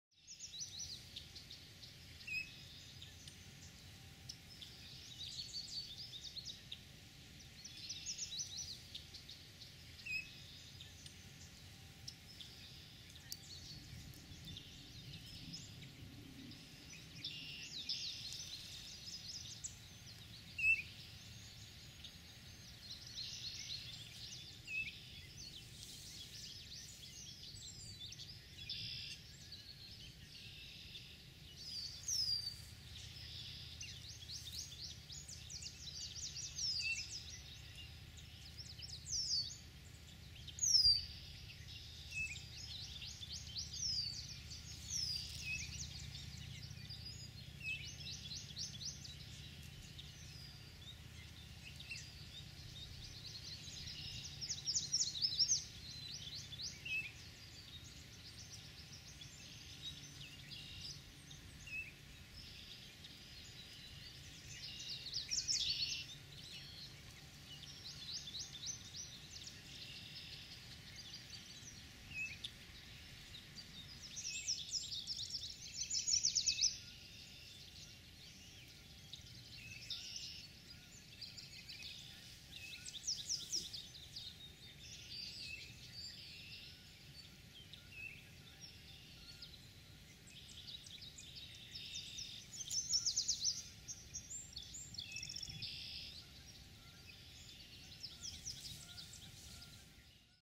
20140510 Parc de la Frayère, Boucherville H264 1080p
ORIOLE DE BALTIMORE
Parc de la Frayère, Boucherville, Qc

La femelle prépare son nid: c'est fascinant de la voir effilocher méthodiquement l'écorce d'une branche!